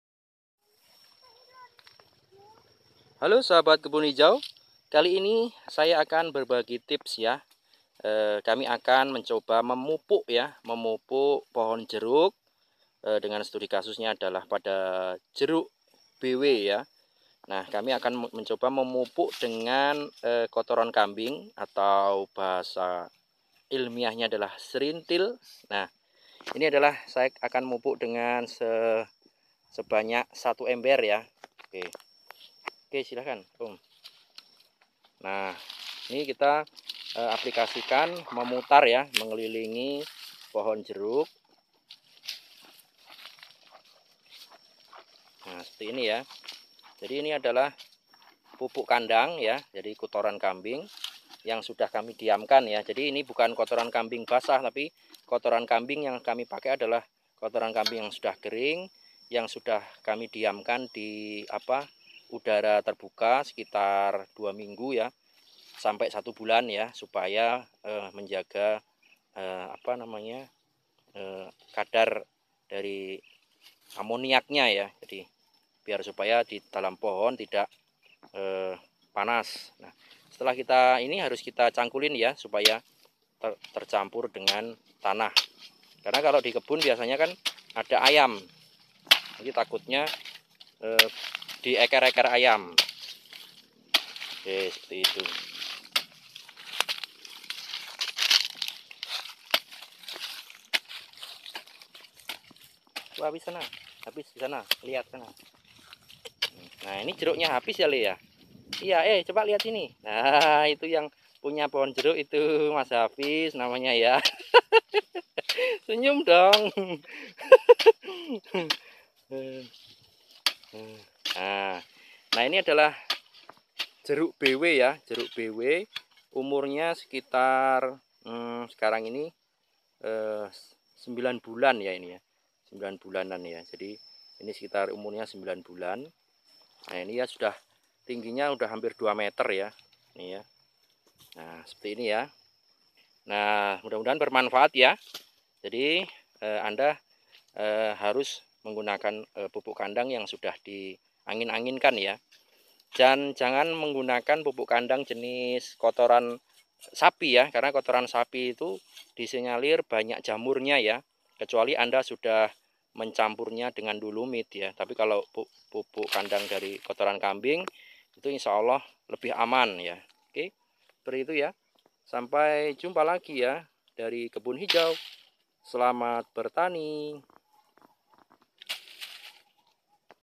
Memupuk Tanaman Jeruk Keprok BW dengan Kohe (Kotoran Kambing)
Memupuk Tanaman Jeruk Keprok BW dengan Kohe (Kotoran Hewan / Kamping)

Jeruk BW berusia kurang lebih 9 - 10 bulan, ditanam di awal tahun 2018 dan saat rekaman pada akhir bulan Oktober 2018

Pada lokasi 1, ada sekitar 70 batang jeruk keprok bw

Lokasi Kebun Hijao ada di Way Jepara, Lampung Timur